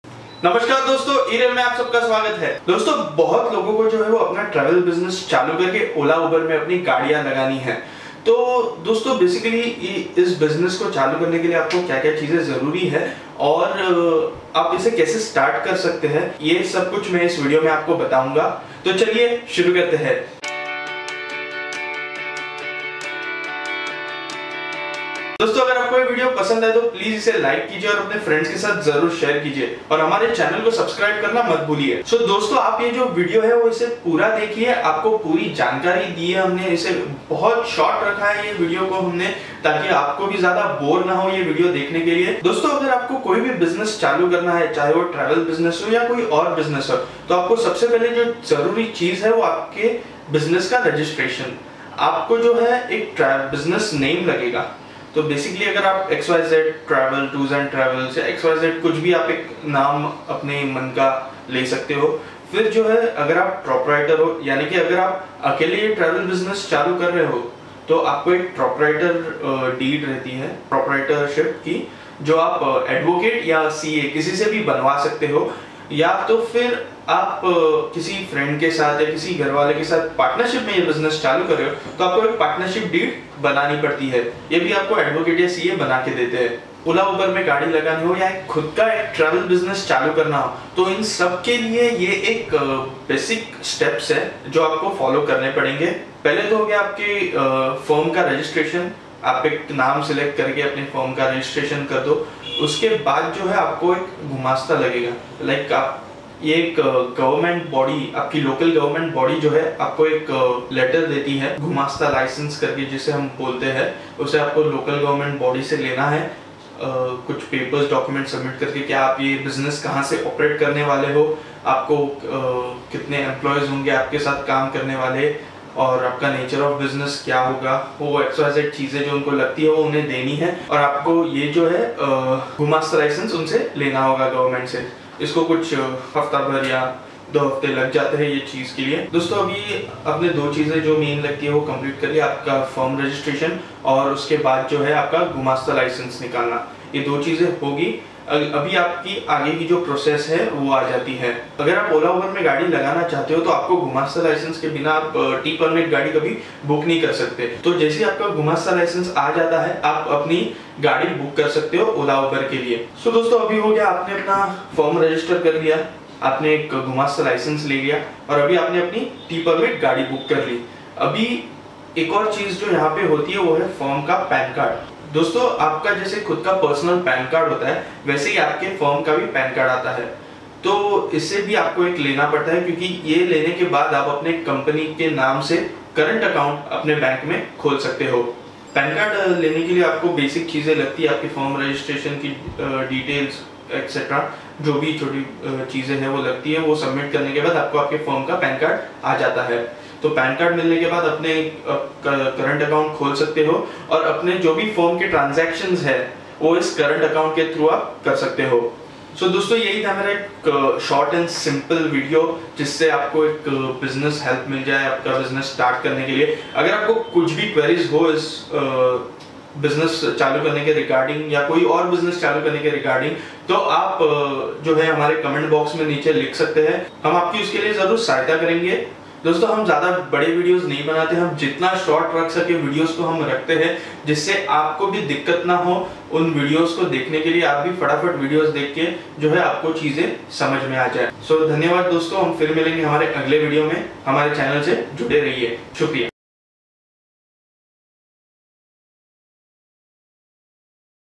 Ola Uber k Sath apna Travel Business kaise start kare? How to start ur own travel business with Ola?
Dost is video mein humne bataya hai k aap apna khud ka travel business kaise start kar sakte hai. Appko kya kya important cheese lagne wali hai ekdam short mein samjaya hai.
Which documents are required to start a travel business?
which papers are required to start ola and uber business?
ola and uber business requirements